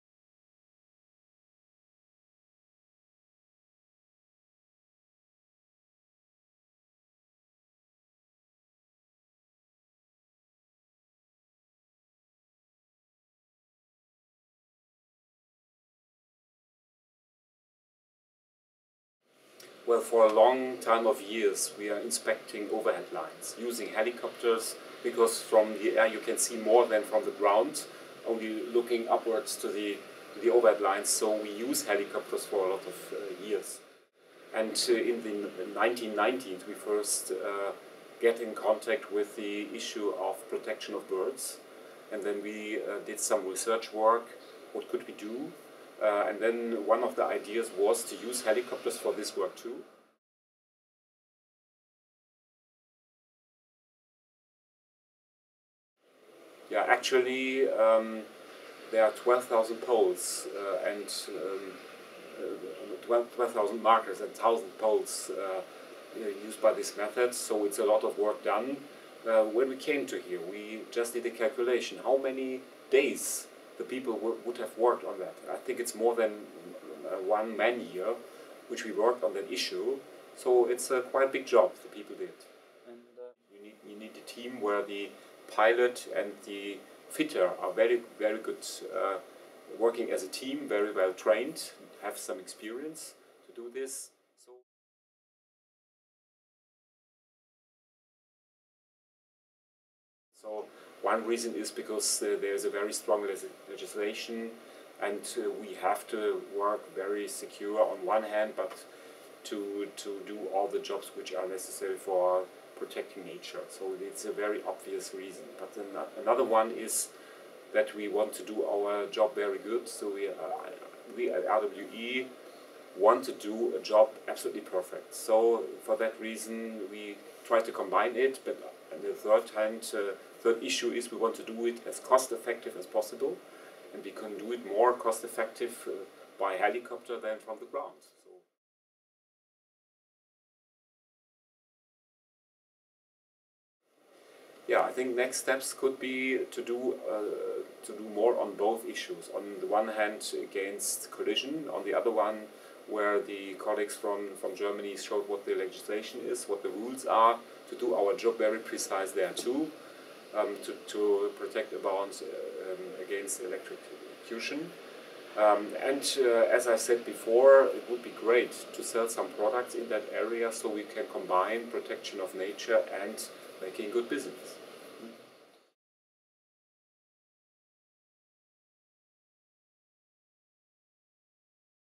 Fitting power lines with bird reflectors - Interview
Dr. Stefan Kueppers, Managing Director of RWE Rhein Ruhr Netzservice GmbH, explains how the company fits power lines with bird reflectors using a helicopter.
The interview was done on occasion of the CMS COP10 in November 2011 in Bergen, Norway.